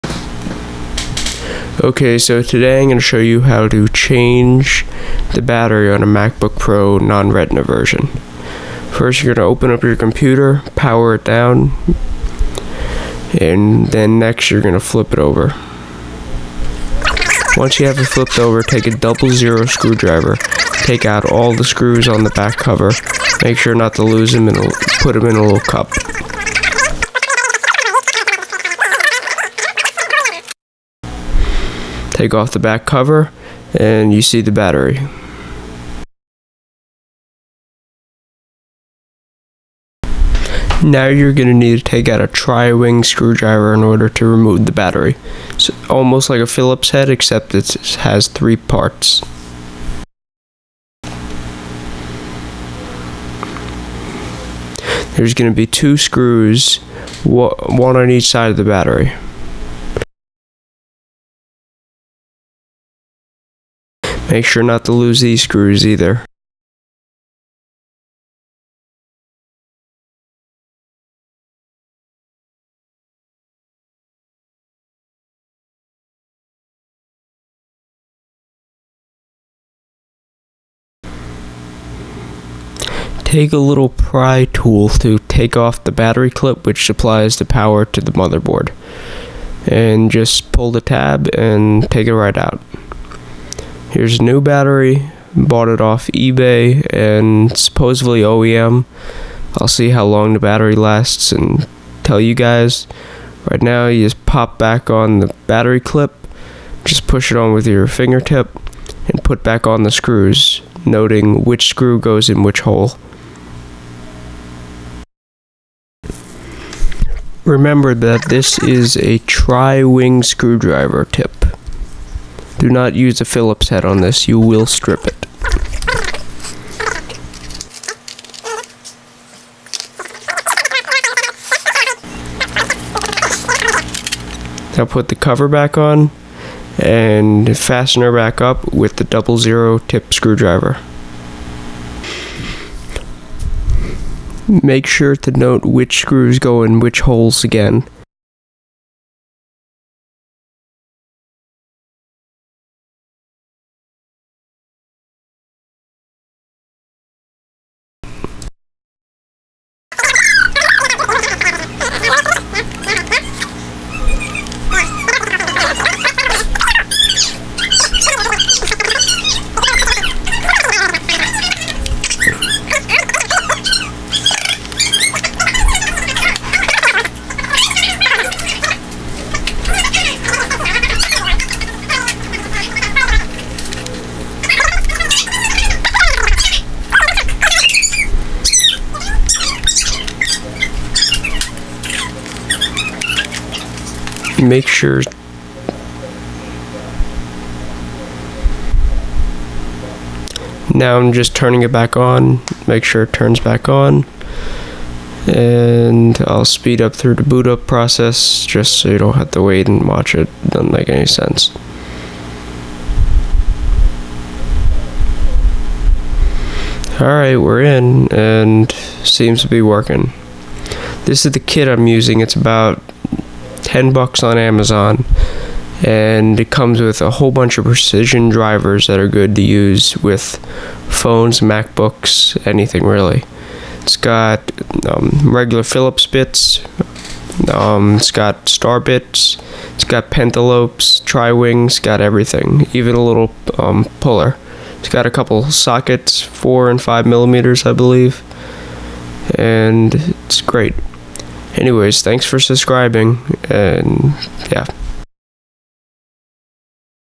How To Change the Battery on a MacBook Pro 13" (Non-Retina Display)
In this video, I explain how to change the battery in my MacBook Pro Mid 2010 model.  This method should be the same for all the uni-body models,  probably doesn't work for the retina models.  I go step by step in this tutorial and explain what tools you need to do the job fast and easy.

This is the tool kit I bought:
-- tool kit

---#00 Philips Head Screw Driver
---Tri 0 Tri-Winged Screw Driver
---A Plastic Prying tool